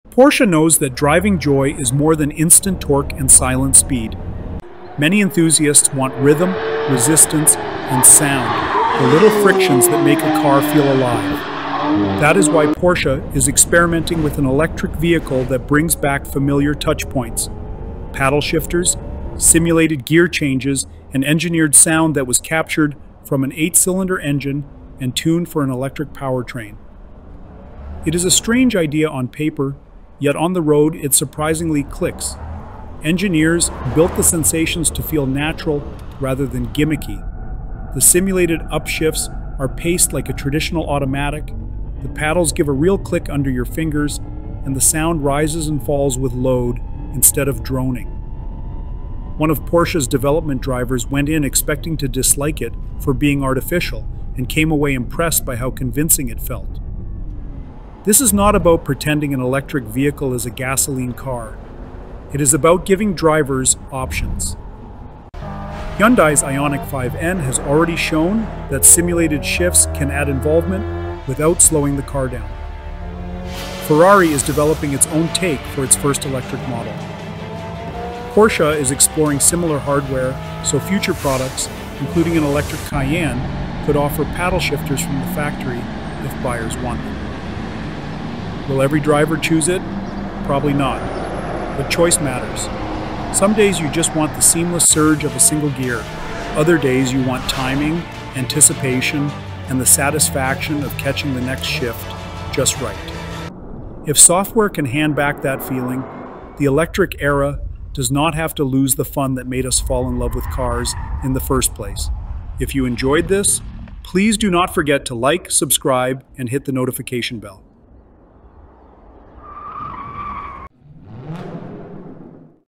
Porsche’s Electric Surprise: Fake Gear Shifts That Feel Real?
Porsche is testing simulated gear shifts, paddle shifters, and engineered sound to bring back feel in its next wave of electric vehicles. It sounds odd, but on the road it just might work.

In this video we show how Porsche builds believable “friction” into an electric powertrain: shift timing, paddle feedback, and sound mapped to load rather than volume. We also compare approaches from Hyundai Ioniq 5 N and why Ferrari is developing similar features. You will see why optional software and simple hardware like paddles could make electric cars more involving without sacrificing performance.

We break down why simulated shifts can boost driver engagement, how sound is captured and retuned from an 8 cylinder baseline, what might arrive on future models like an electric Cayenne, and the tradeoffs for purists versus everyday drivers. Let us know if you would choose it or switch it off.